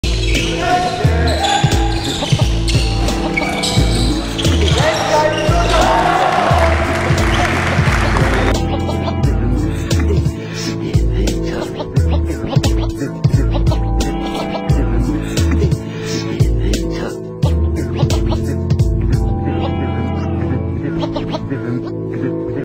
monster dunk P.Bochenkiewicz (WKK)
AWF Katowice vs WKK Wrocław, sezon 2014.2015, 2L gr.C